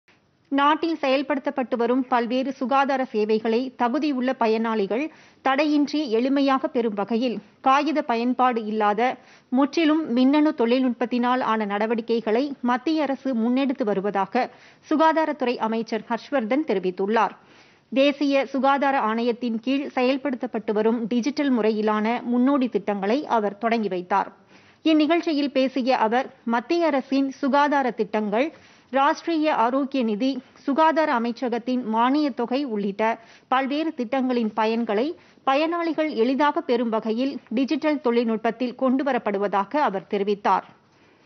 டிஜிட்டல் முறையிலான சுகாதாரத்திட்டங்கள்
நாட்டில் செயல்படுத்தப்பட்டு வரும் பல்வேறு  சுகாதார  சேவைகளை  தகுதி உள்ள பயனாளிகள்    தடையின்றி எளிமையாக  பெறும் வகையில்,  காகிதப் பயன்பாடு இல்லாத முற்றிலும் மின்னணு தொழில்நுட்பத்திலான  நடவடிக்கைகளை மத்திய அரசு முன்னெடுத்து வருவதாக சுகாதாரத்துறை அமைச்சர் ஹர்ஷ்வர்தன் தெரிவித்துள்ளார்.